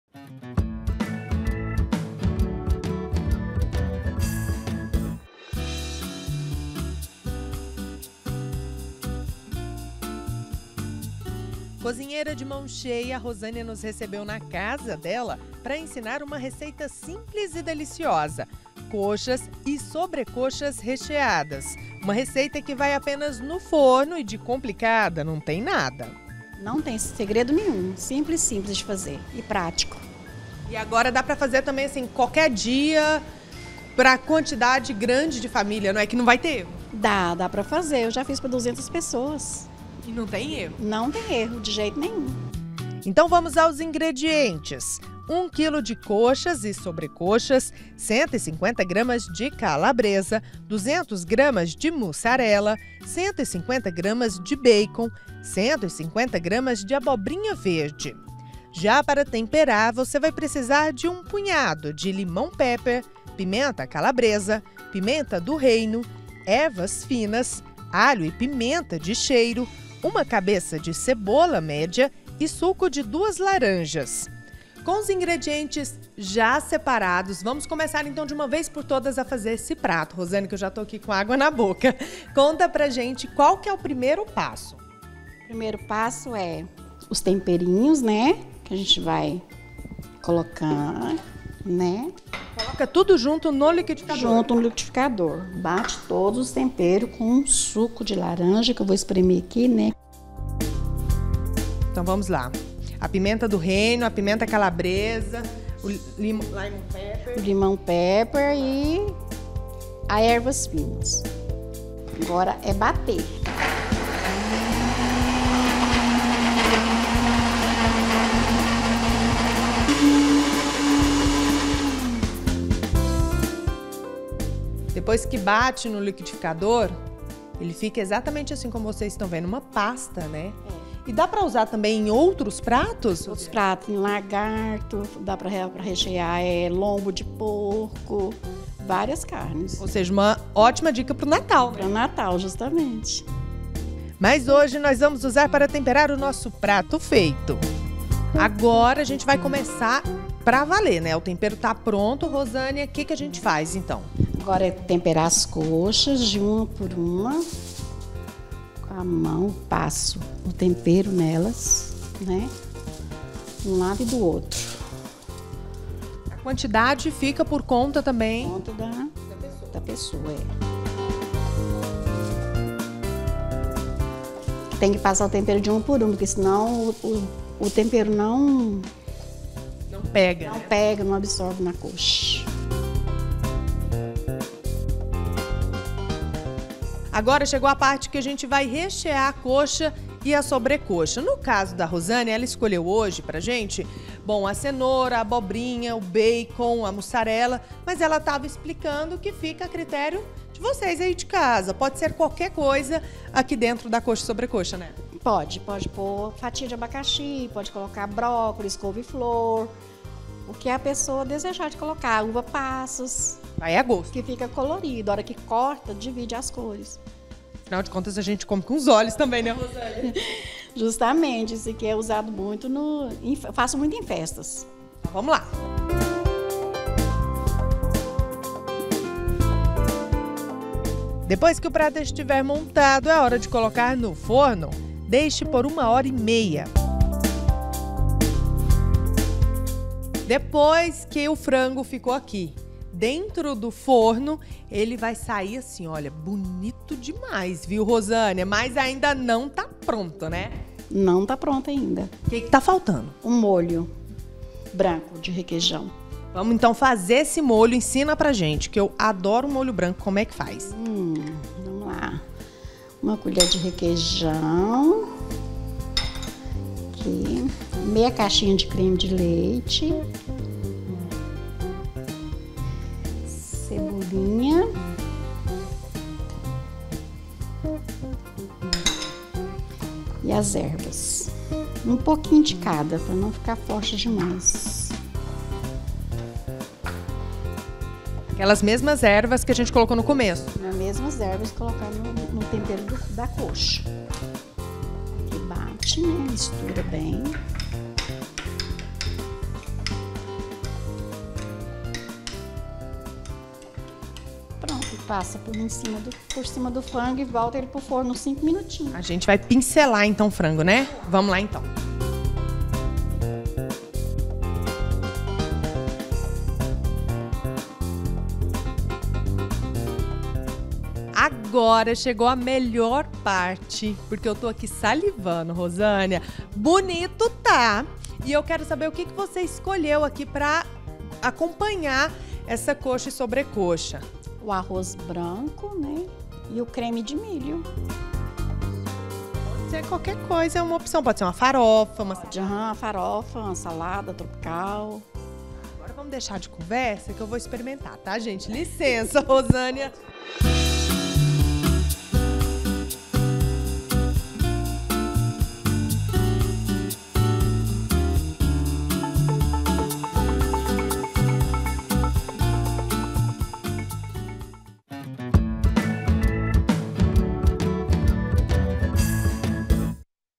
PRATO FEITO: COXA DE FRANGO RECHEADA AO CREME
NO PRATO FEITO DE HOJE, VAMOS TE ENSINAR A DAR UMA TURBINADA NO FRANGO ASSADO DO FIM DE SEMANA. A MICHELI ROSA FOI APRENDER A FAZER UMA RECEITA DE COXA E SOBRECOXA RECHEADA QUE AINDA É GRATINADA COM UM MOLHO SUPER ESPECIAL.